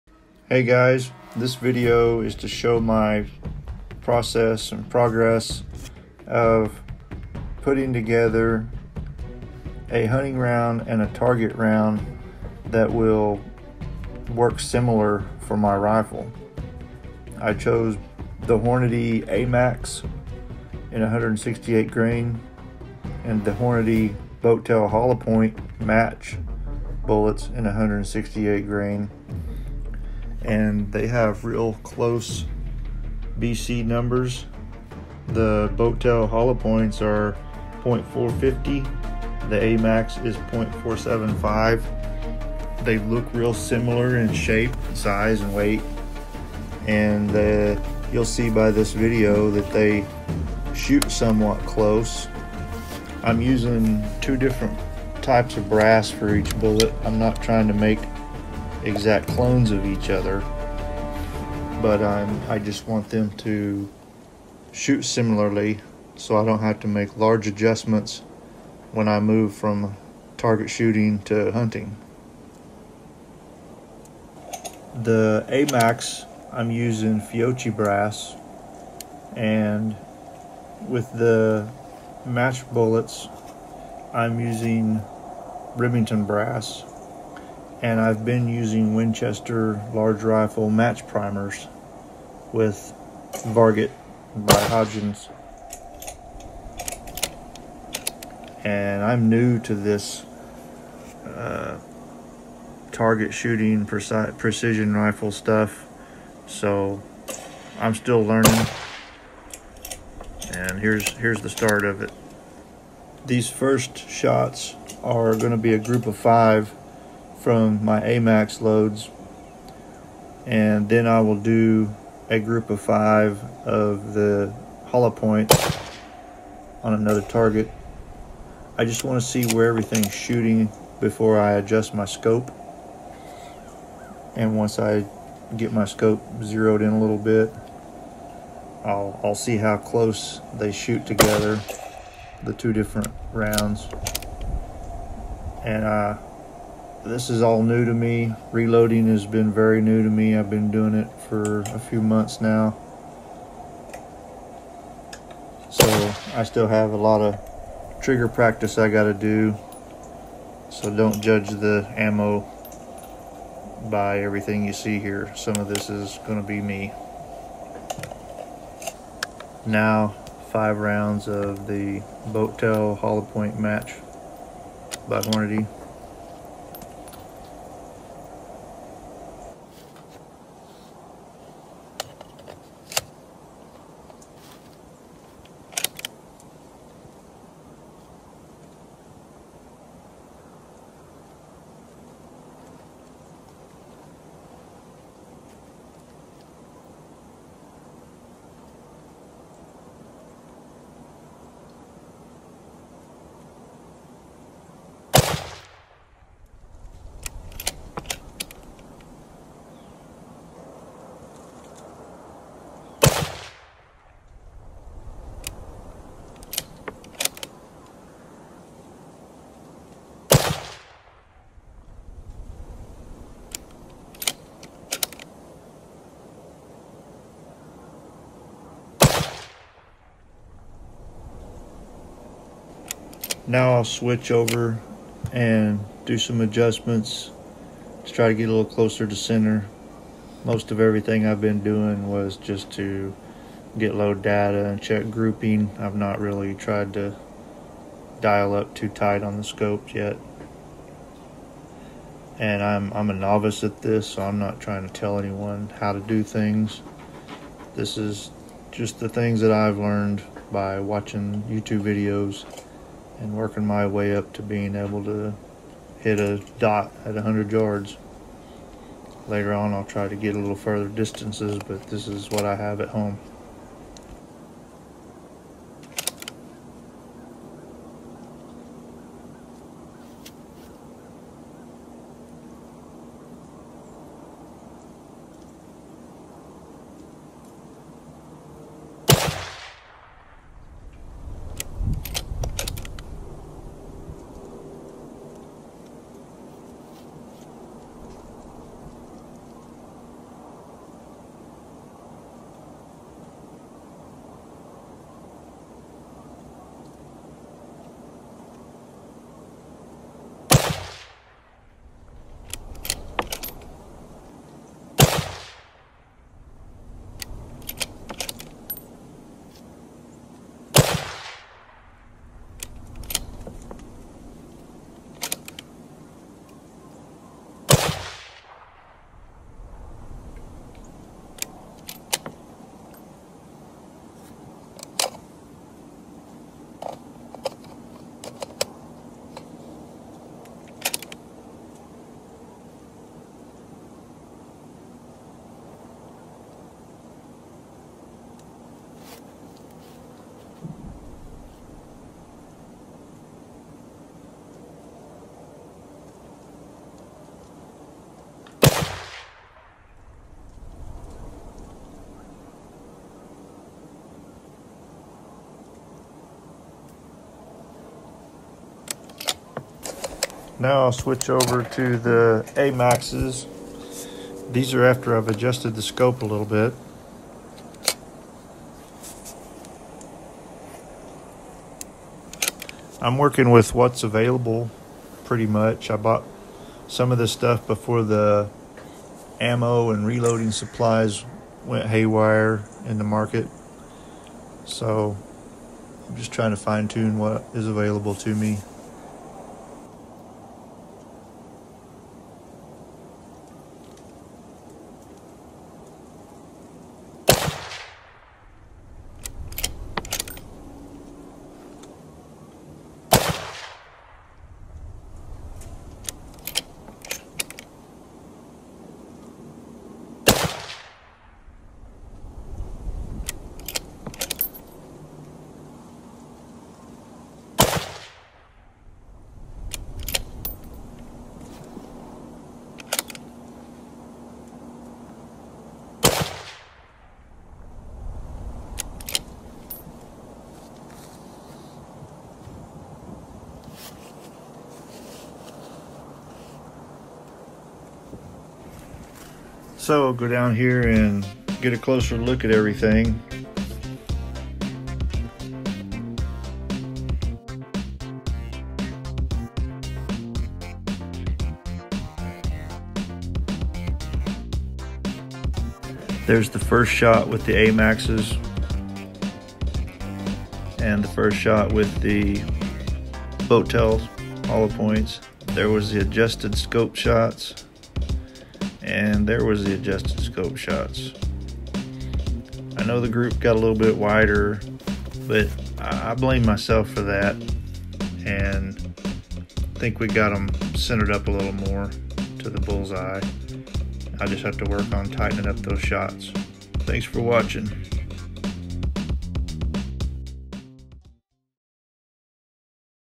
Making target and hunting ammo for the same rifle
My novice journey to the world of reloading and precision rifle.
This was a thrown together video and it was cold outside lol.
When I have more time to sit still and shoot tighter groups I’ll update a video,
I do have tighter groups on my ladder test targets when I was developing these loads so don’t worry I know these groups suck.